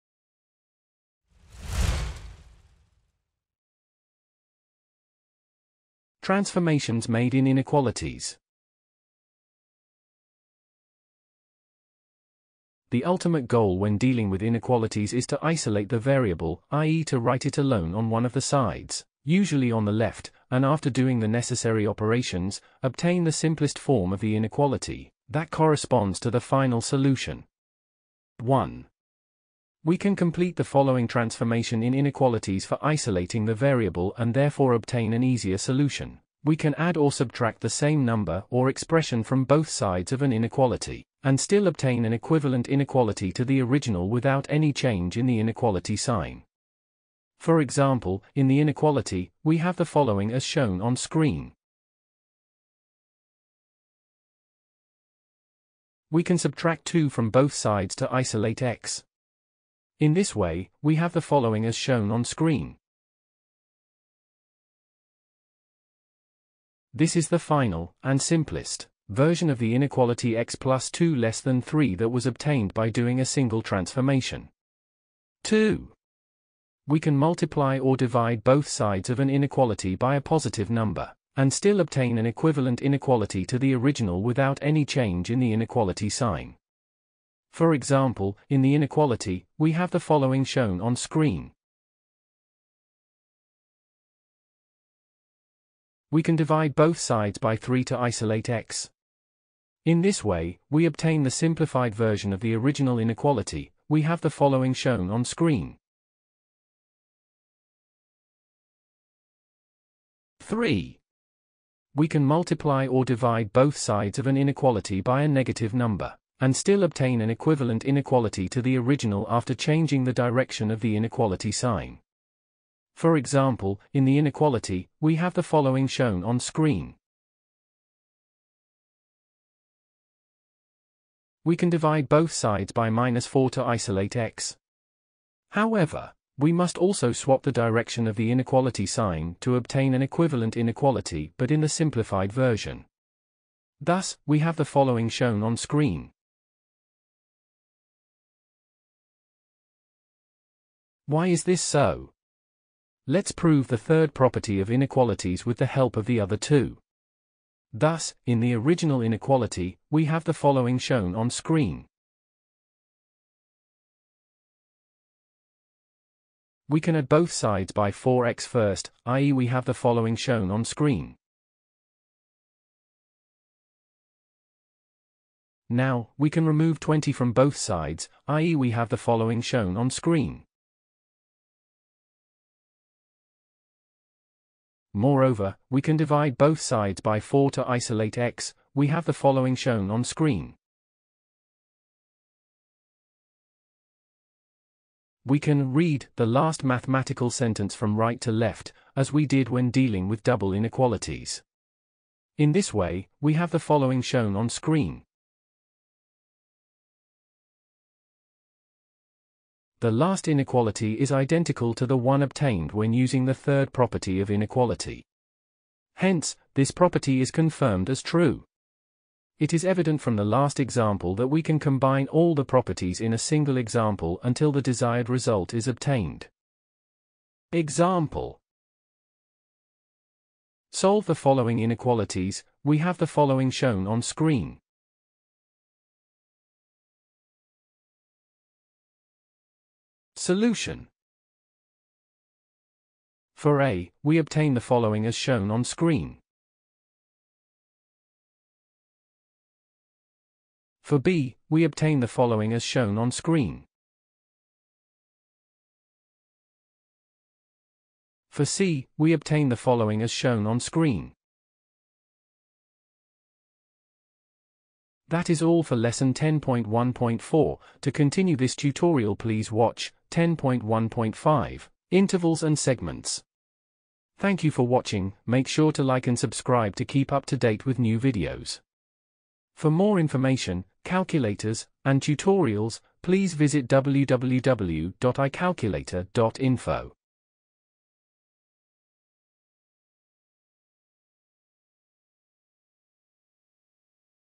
10.1.4 - Transformations made in Inequalities
Welcome to our Math lesson on Transformations made in Inequalities, this is the Fourth lesson of our suite of math lessons covering the topic of Inequalities.